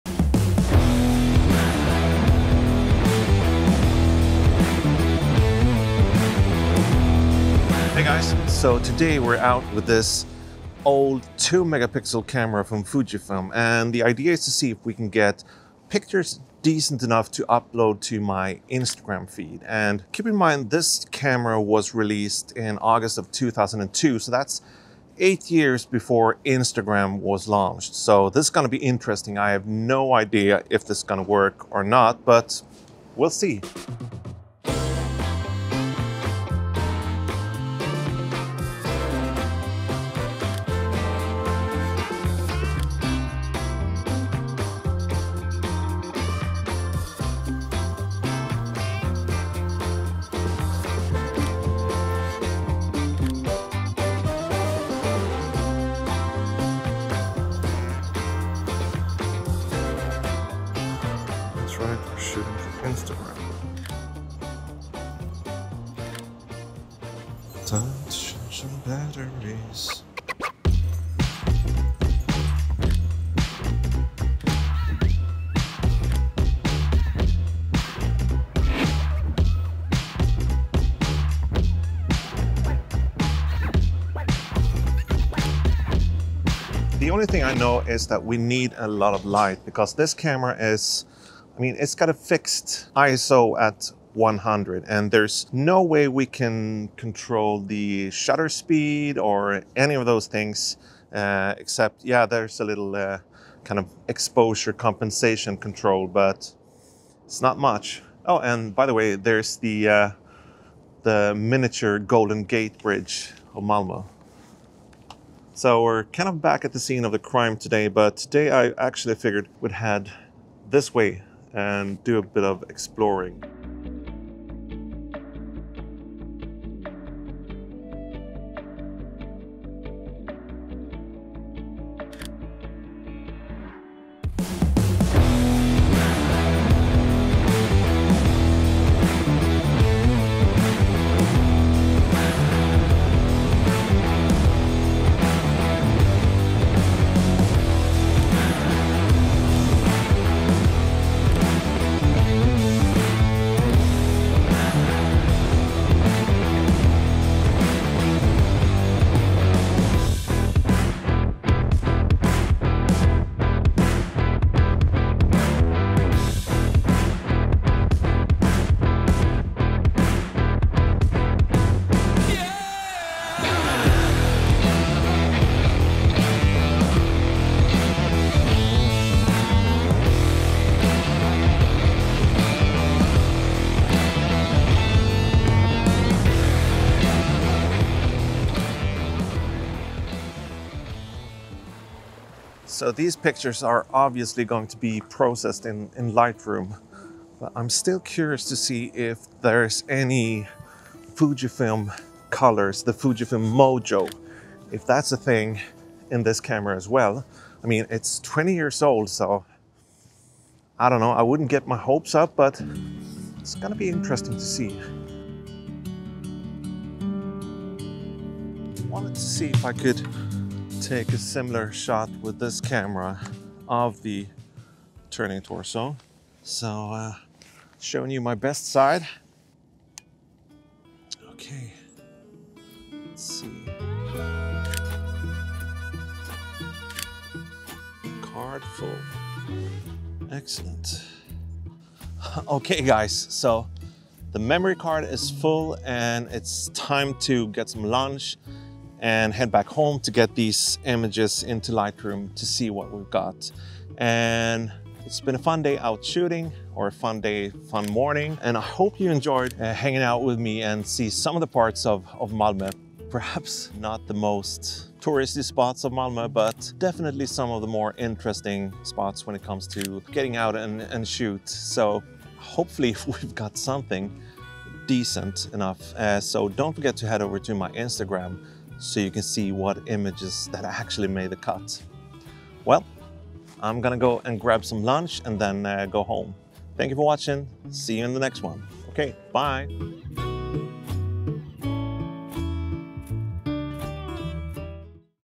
Retro Camera - Good enough for Instagram?
Can you use a 20 year old 2 megapixel camera for instagram shots today?
I just took my old Fujifilm Finepix A204 out for a spin in my hometown Malmö to find out.

and say hi